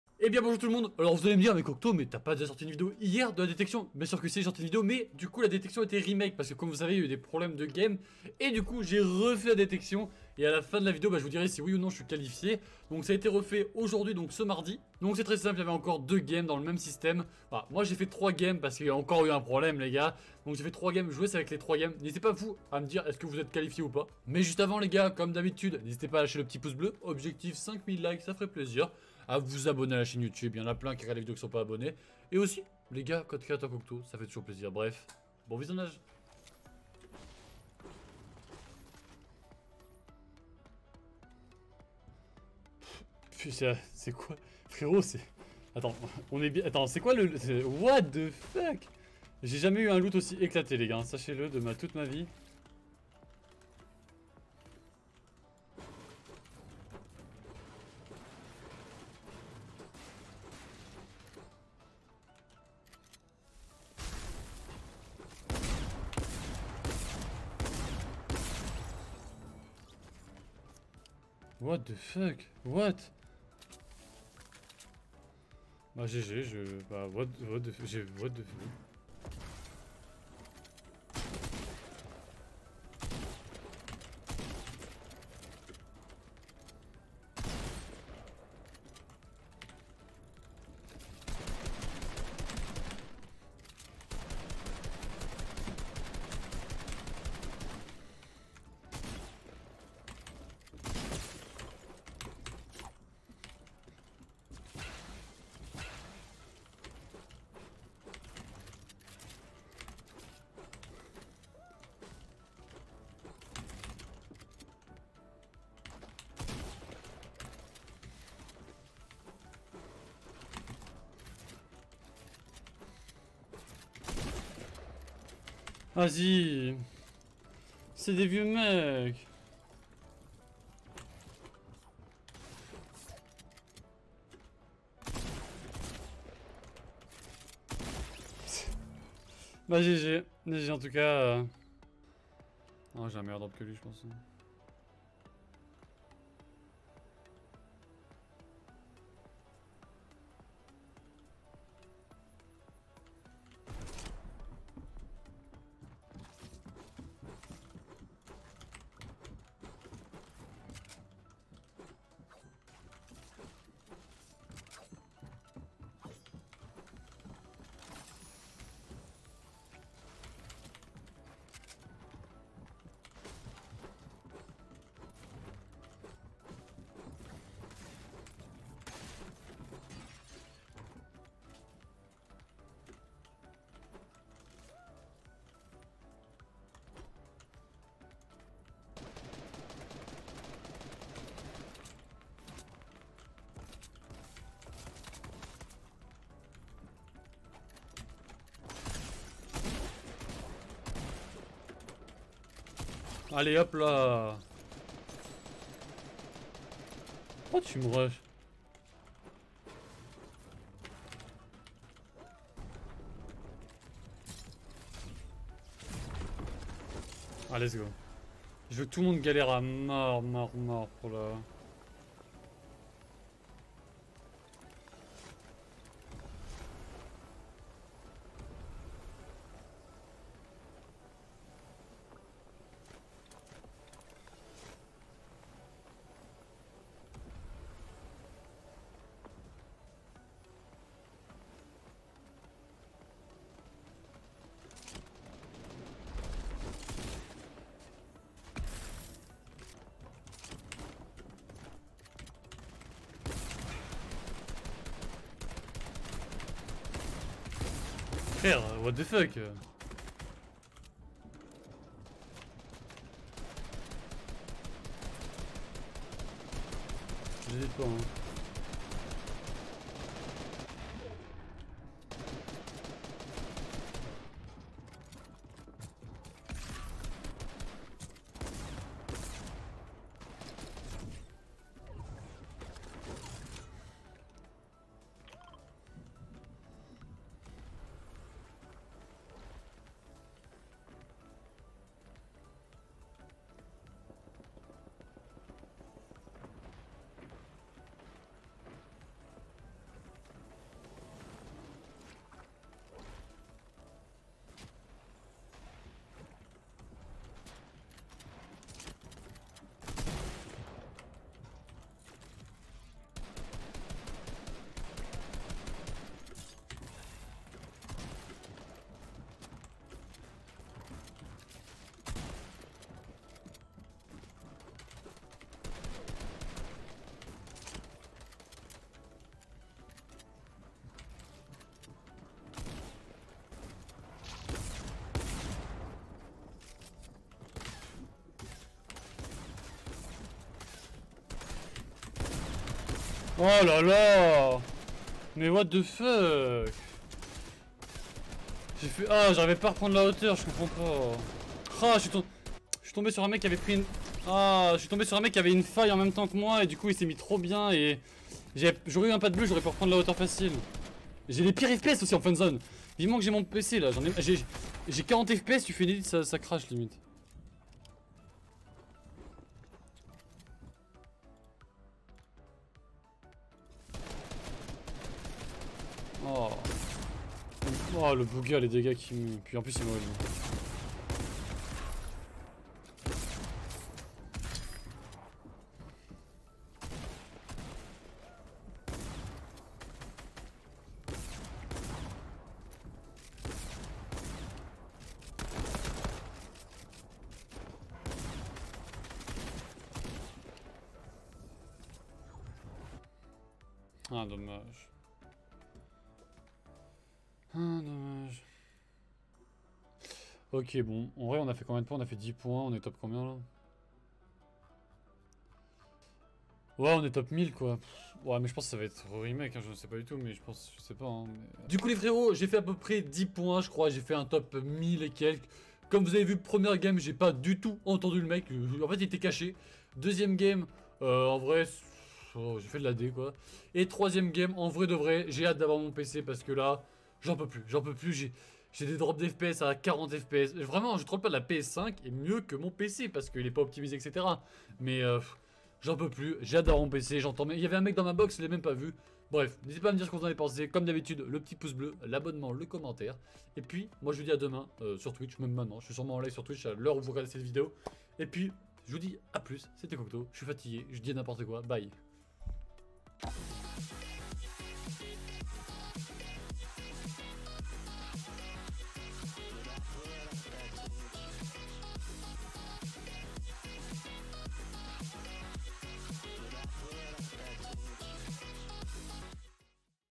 DOIGBY ME DONNE UNE SECONDE CHANCE POUR ME QUALIF A LA DETECTION (Voila le résultat)
J'ai une nouvelle chance de me qualif à la detection de doigby